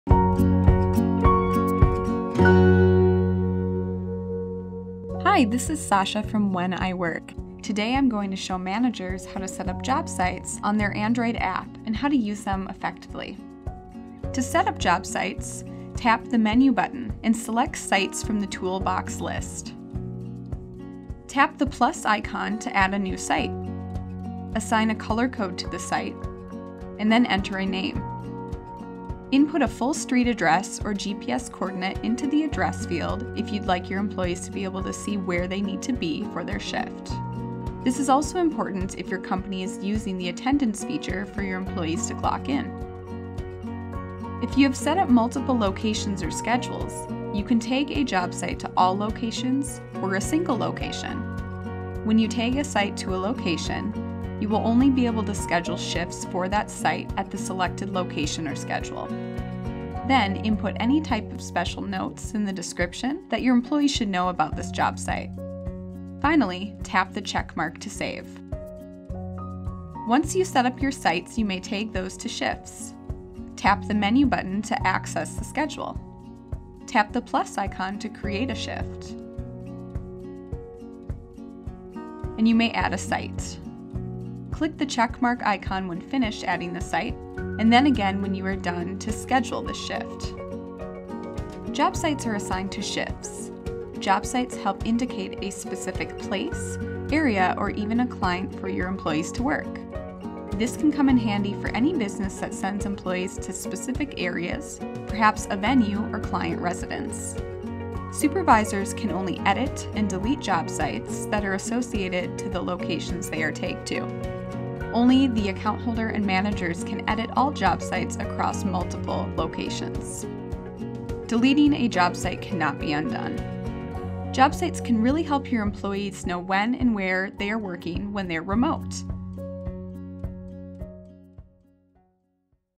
When I Work - Job Sites on Android
Job Sites are used if you schedule employees for shifts that are outside of the office or store. A Job Site might also reflect additional detail that you want to tag to a group of shifts.